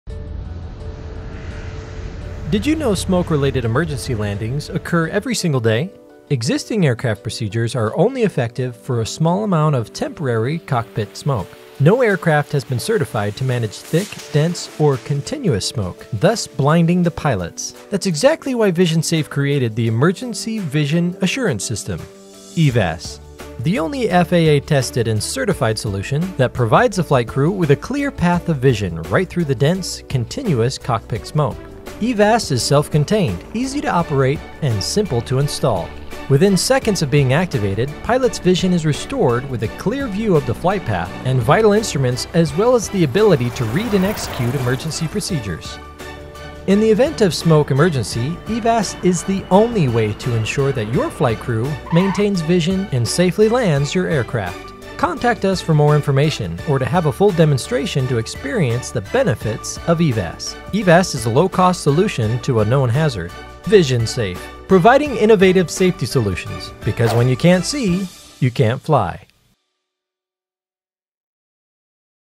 EVAS Commercial Animation
A brief animation detailing the benefits of the EVAS system